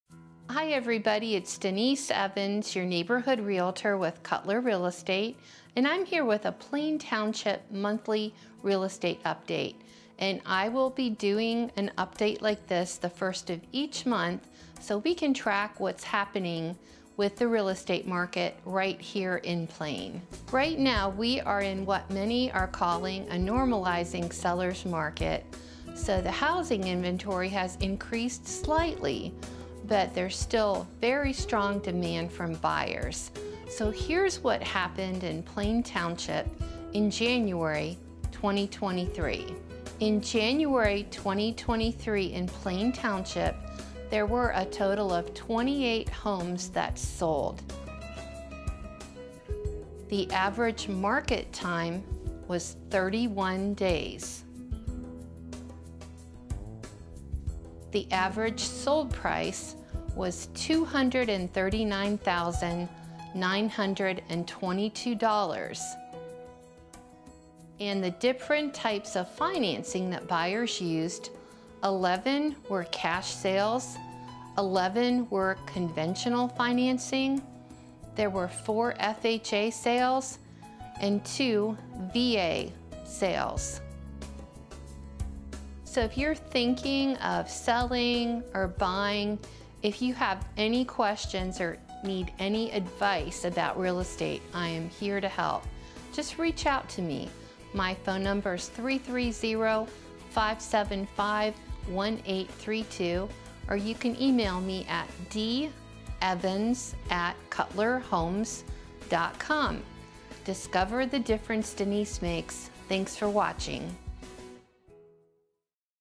Plain Township Canton, Ohio Real Estate Market update Jan. 2023, by Denise Evans, Cutler Realtor
Watch this short video to see how many homes sold in Plain Township, Canton, Ohio in January 2023. You'll also learn the average Days on Market, average Sales Price, and different types of financing that buyers used to purchase their homes. The real estate market is changing, but buyer demand is still very strong, we still have a housing inventory shortage, and 2023 is expected to be a good year for both sellers and buyers. Discover the Difference Denise Makes!